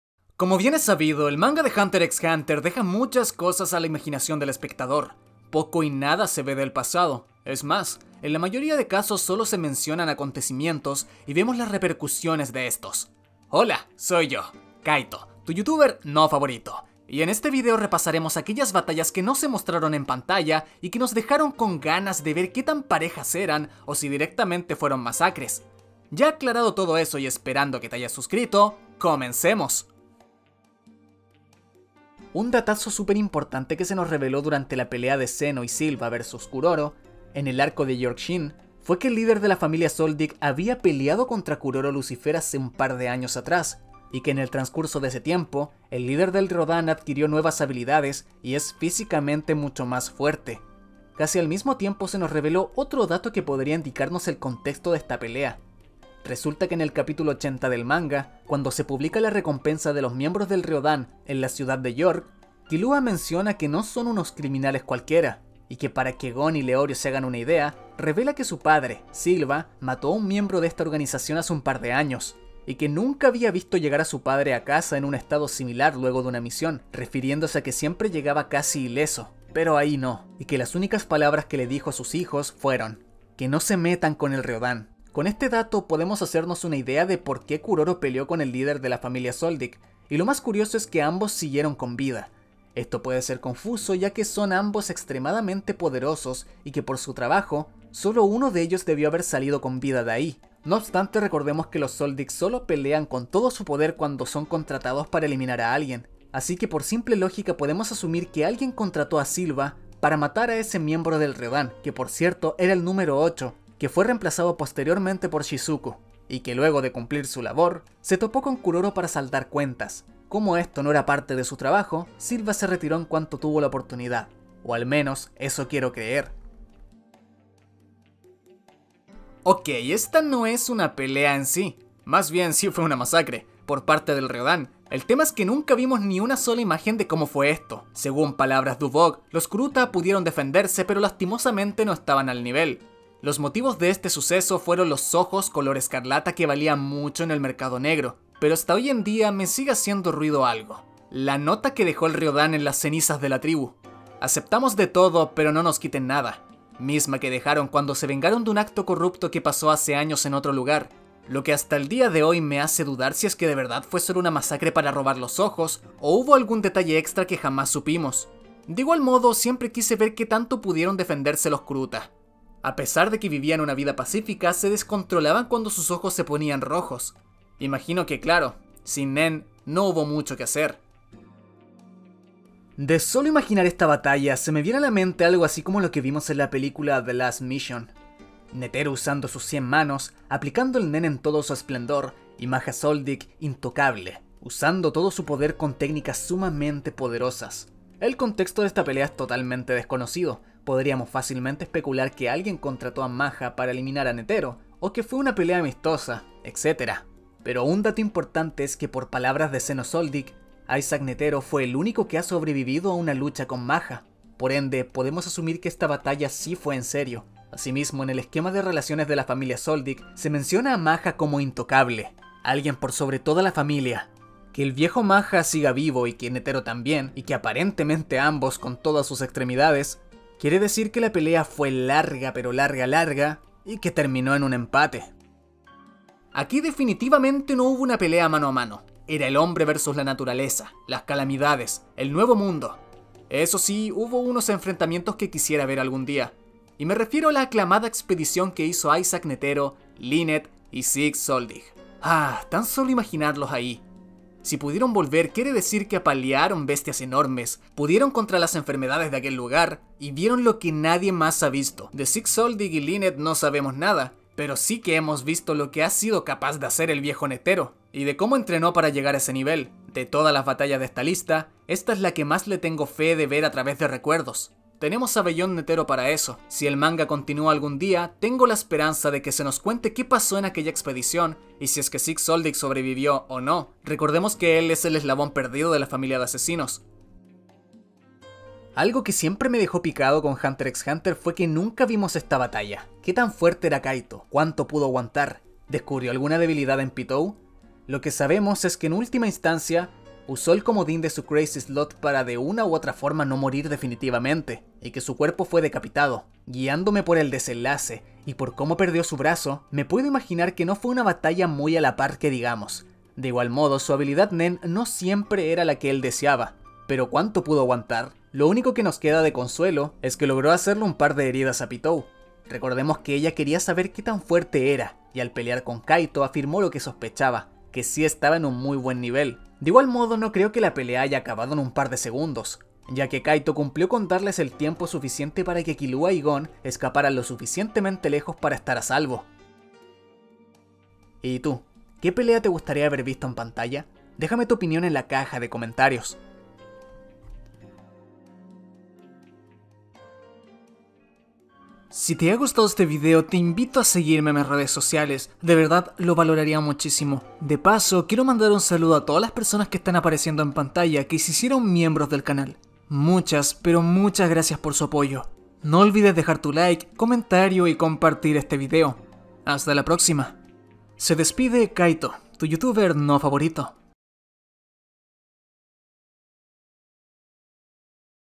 HUNTER X HUNTER | LAS BATALLAS QUE NO SE MOSTRARON
amigos mios! aqui les traigo un video sobre todas las batallas que no vimos en hunter x hunter pero que si pasaron, como la gran pelea entre neferpitou y kaito, o algunas que sucedieron en el pasado, expediciones al continente oscuro, maha zoldyck vs netero, silva zoldyck vs chrollo